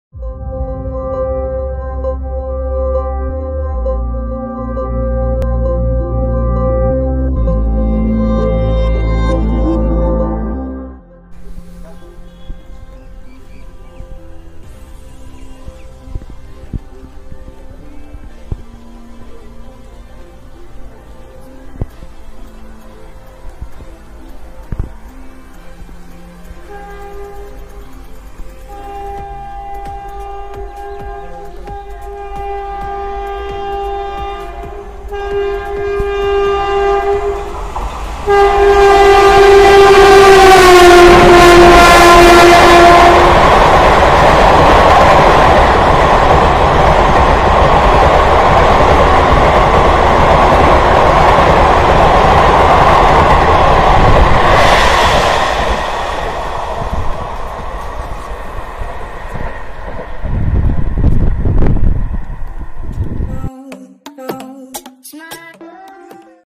TREMENDOUS AGGRESSION OF BALURGHAT - KOLKATA SPECIAL || INDIAN RAILWAYS
BALURGHAT - KOLKATA TEBHAGA EXPRESS
LOCO:- 22377 HWH WAP-4  + 11204 HWH WDM3A